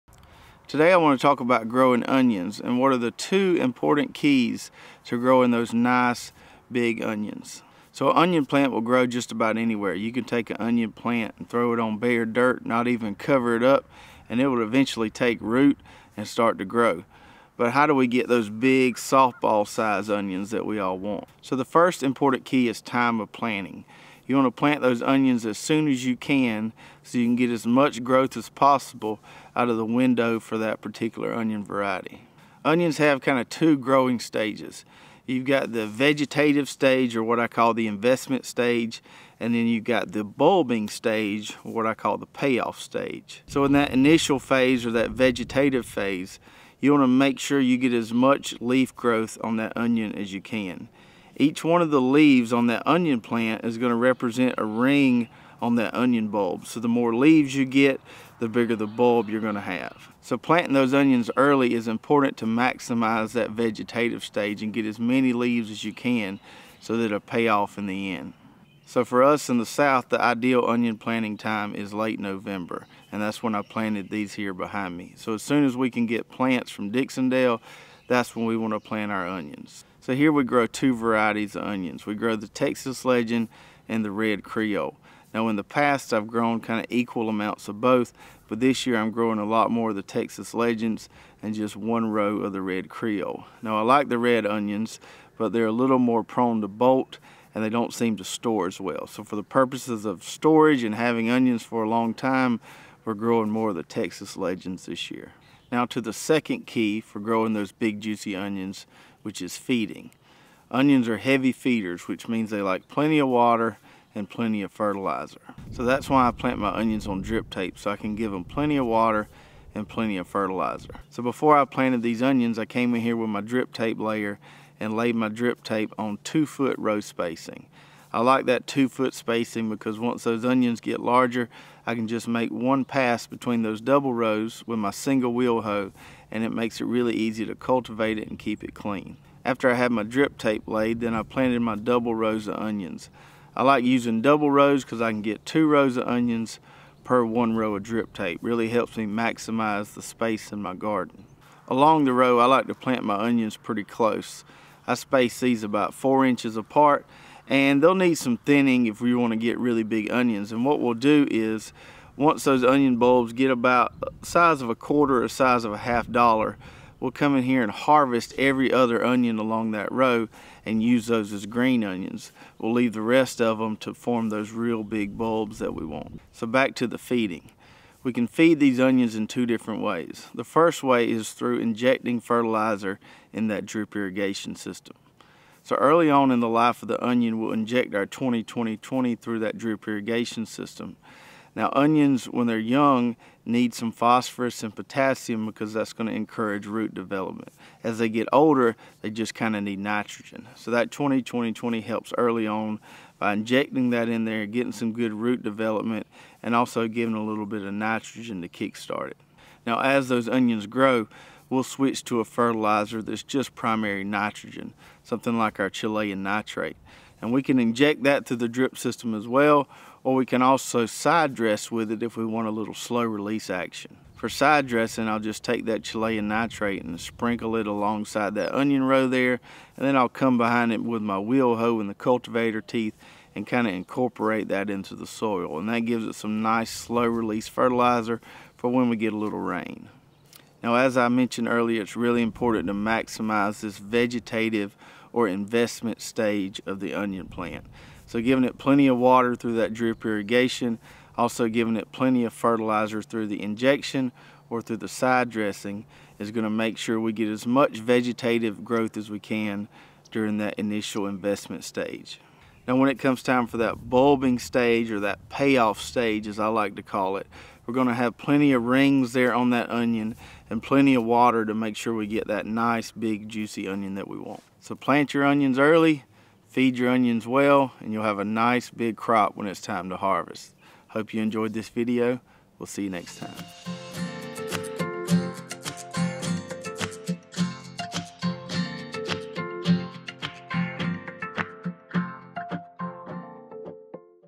The 2 Most Important Tips for Growing Big Onions
Onions will grow just about anywhere, but what are the keys to growing the big, softball-sized onions that everyone wants?

0:00 Start
0.23 First Important Tip
0:36 Two Growing Stages Of Onions
1:49 Second Important Tip For Growing Big Onions
2:05 Laying Drip Tape
2:36 Planting Onion Plants